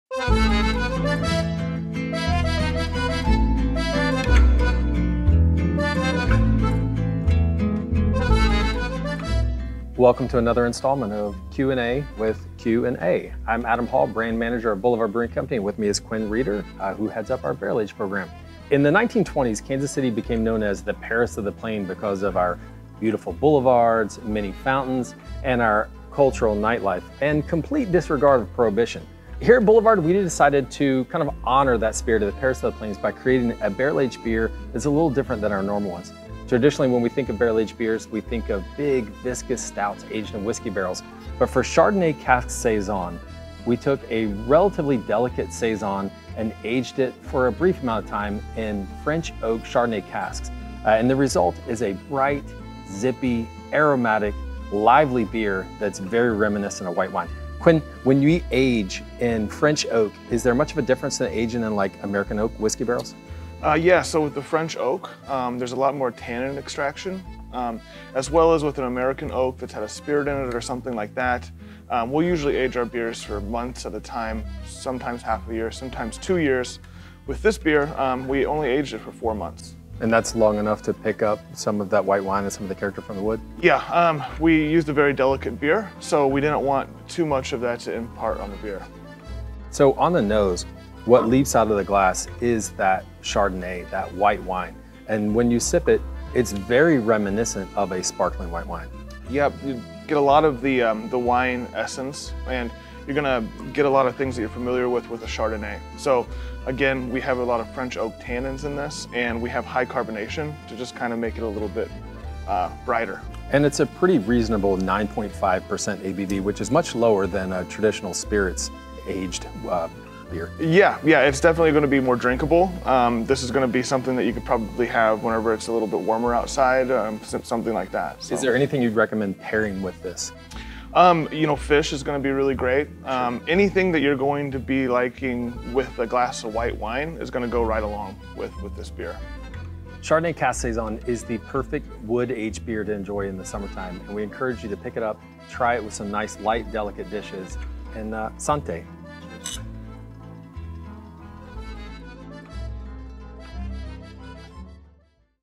Q & A with Q & A Chardonnay Cask Saison
Boulevard Brand Manager Adam Hall and Brewer / Barrel Manager Quinn Reeder discuss our summer 2024 limited release Chardonnay Cask Saison.